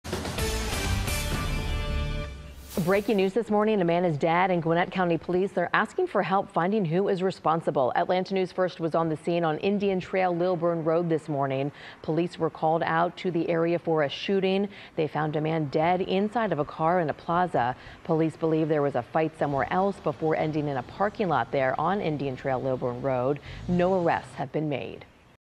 Man found dead in car at Gwinnett County shopping center, police say
A man was found dead inside a car at a shopping plaza in Gwinnett County early Sunday morning, police said.